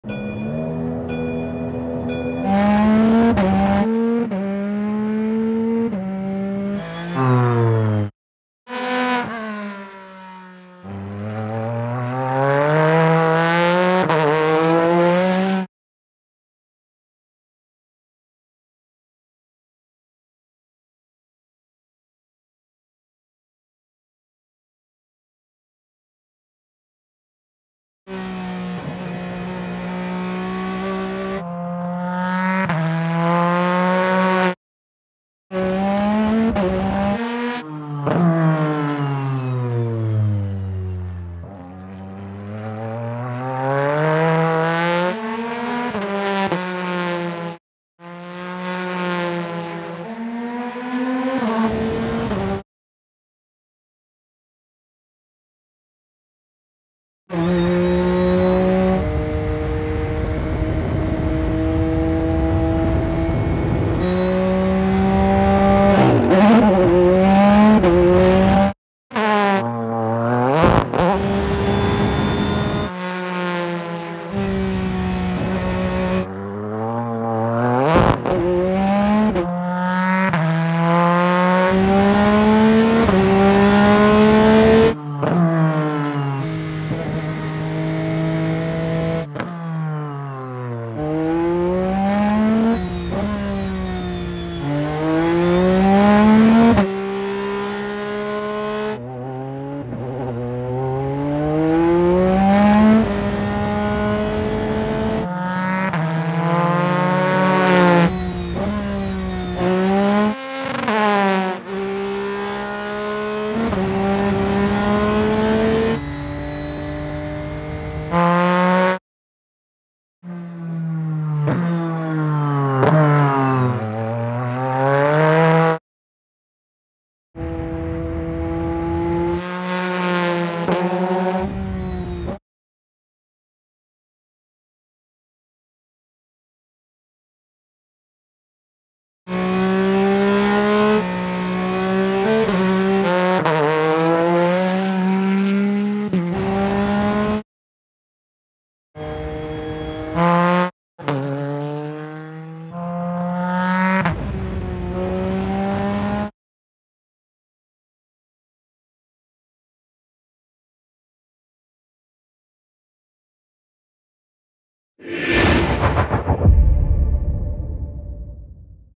The new 911 RSR - driving footage
Its edge? Over 30,000 race victories in 70 years. Its drive? Our team of engineers, mechanics, drivers and fans all over the world. The new 911 RSR is the 911 for the greatest challenges. Take a look at how the 911 RSR drives on track without any soundtrack in this video.